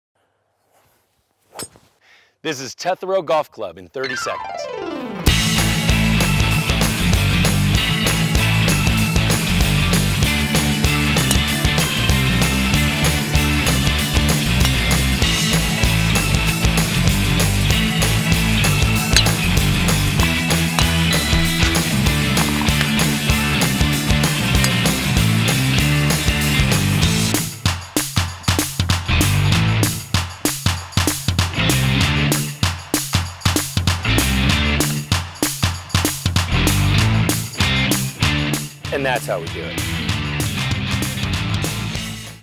Tetherow - 18 Holes in 30 Seconds
Experience all 18 holes of Tetherow - plus a beer break - in just 30 seconds.